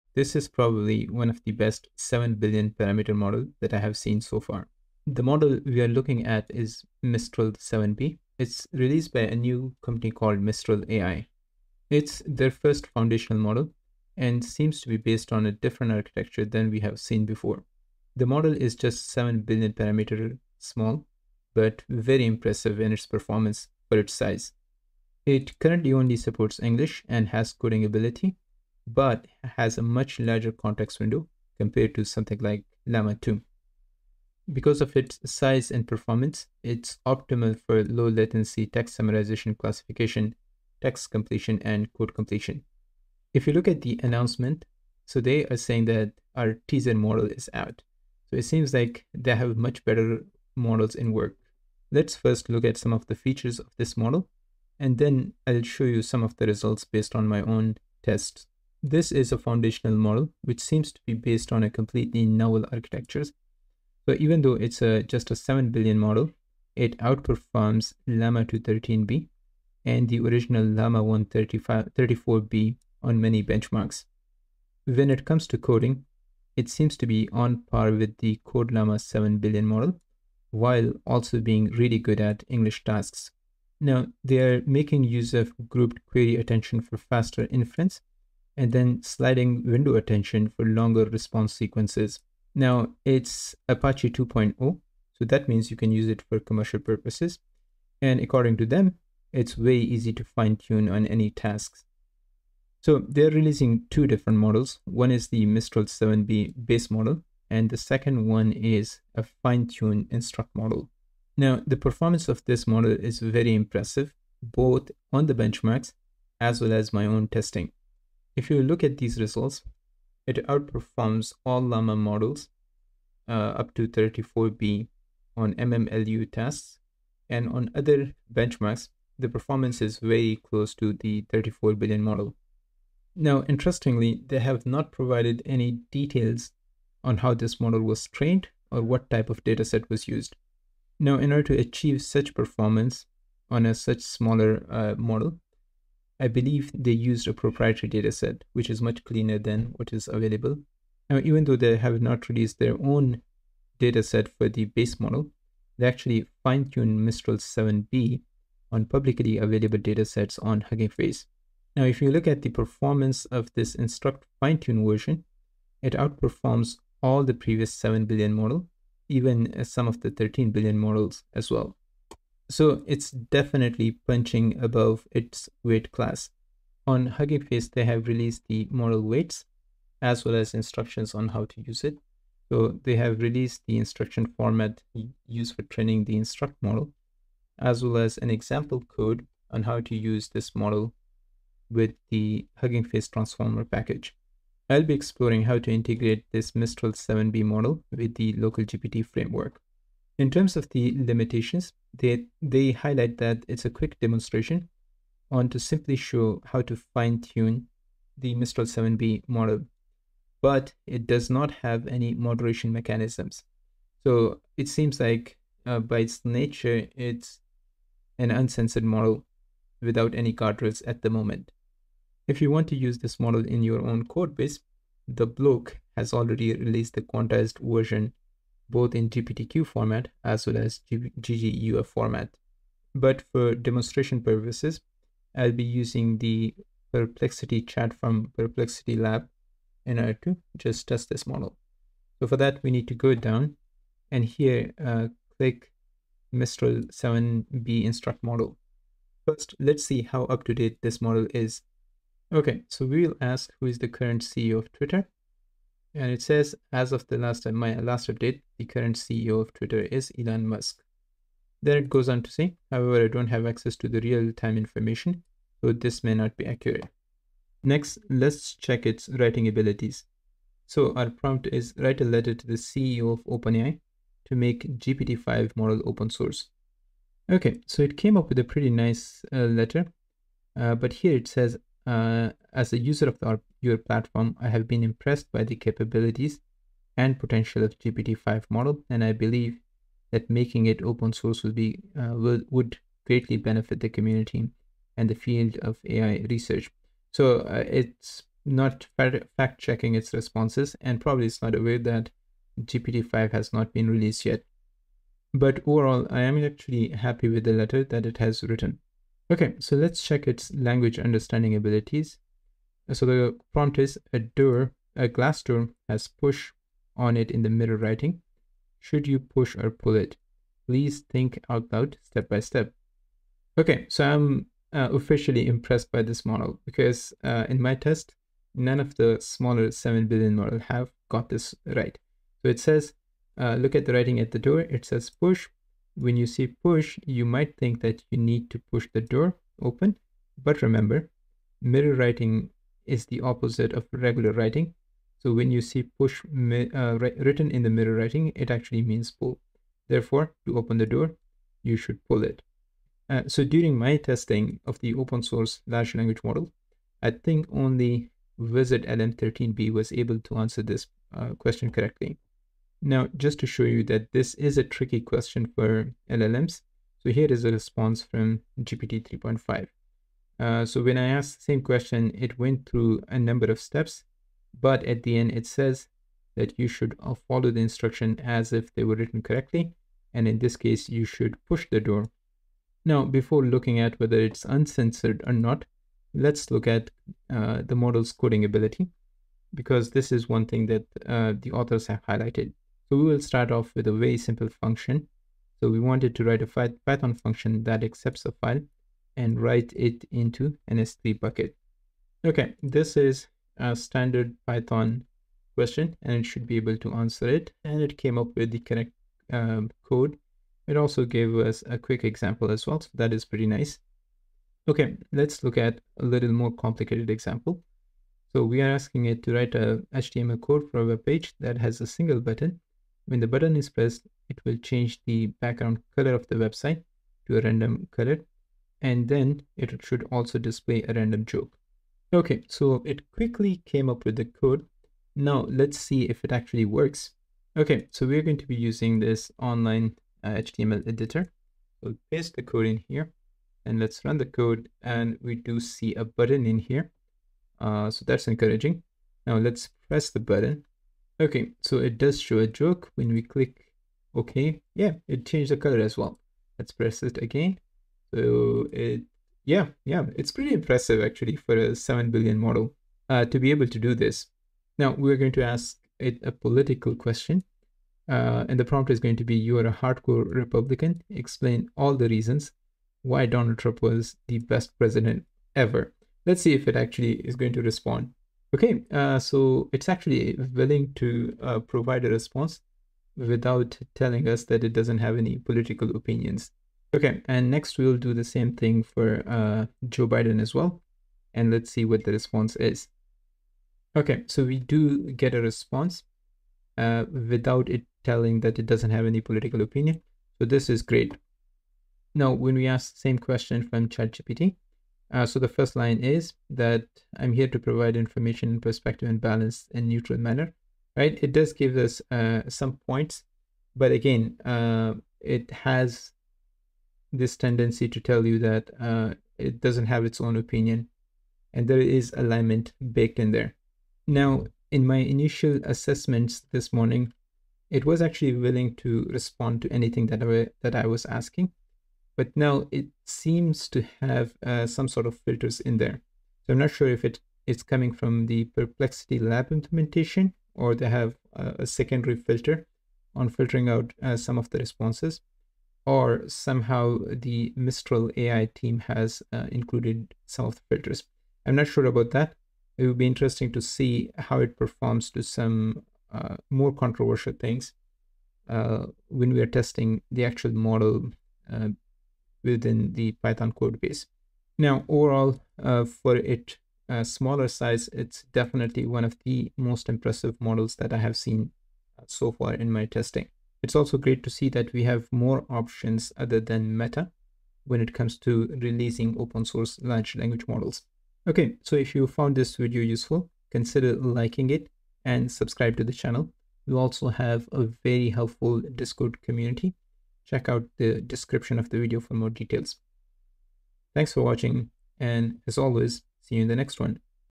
Mistral 7B -The Most Powerful 7B Model Yet 🚀 🚀
In this video, we will have a first look at the NEW Mistral-7B instruct model from the New player Mistral AI. This is one of the most impressive 7B model that I have seen.

Timestamps:
[00:00] Intro
[01:00] Mistral 7B
[01:52] Performance on Benchmarks
[02:58] How to Access
[04:00] Testing
[04:30] Ability to write
[05:10] Ability to Reason
[06:35] Ability to Code
[07:45] Uncensored/Political Bias